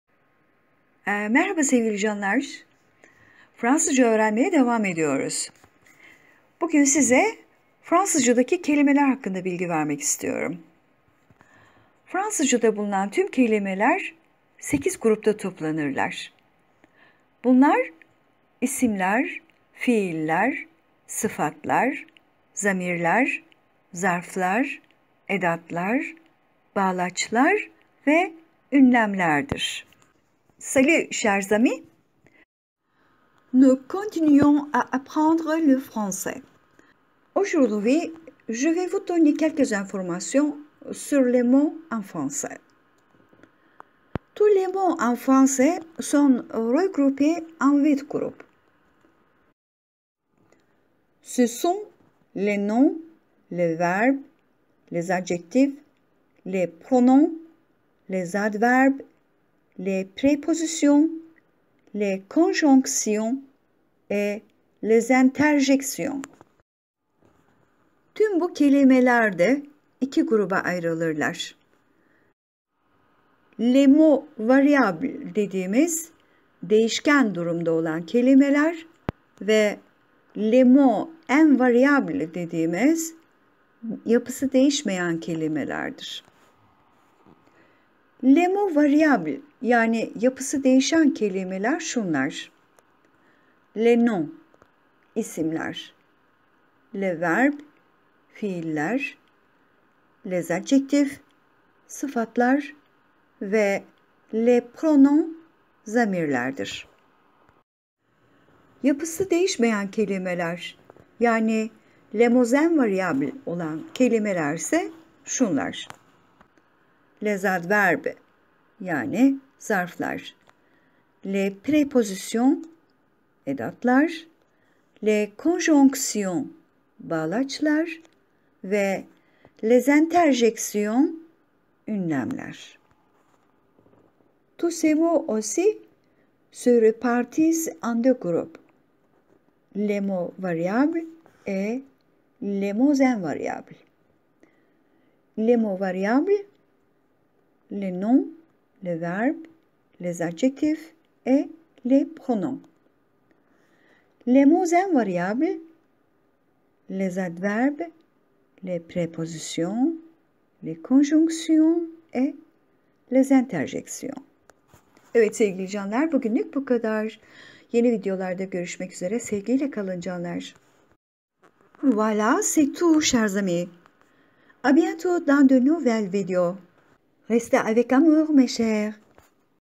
FransızcadaKelimeÇeşitleri, VaritionsGrammaticalesdesMotsenFrançais
Fransızcada, sözcüklerin dilbilgisi bakımından çeşitleri nelerdir, bu videoda bunları öğreneceksiniz. İyi çalışmalar dilerim.
 (Dans ce video, vous allez apprendre "Quelles sont les variations grammaticales des mots en français ?")
Bonne apprentissage !